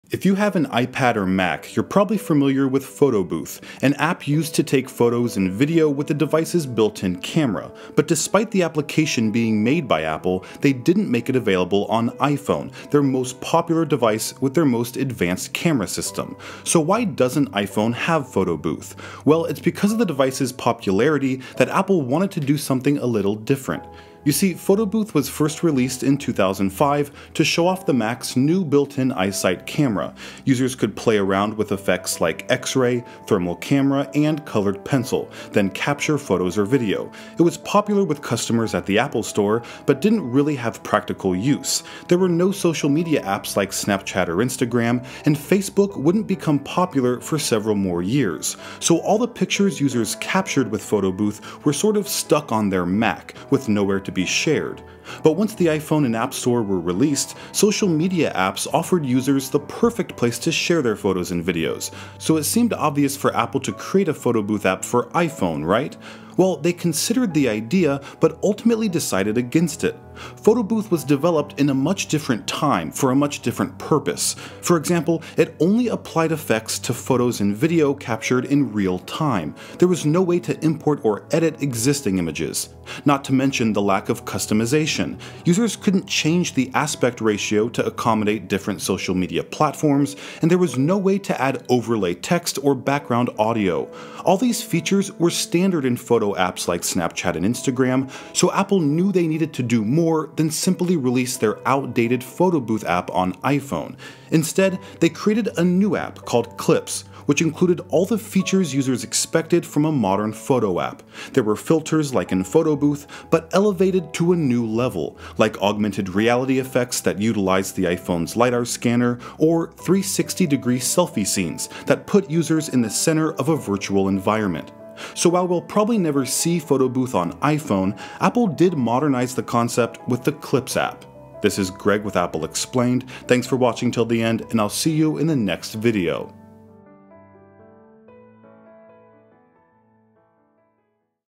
Why iPhones Don't Have Photo Booth
If you have an iPad or Mac, you’re probably familiar with Photo Booth. An app used to take photos and video with the device’s built-in camera. But despite the application being made by Apple, they didn’t make it available on iPhone. Their most popular device with their most advanced camera system. So why doesn’t iPhone have Photo Booth?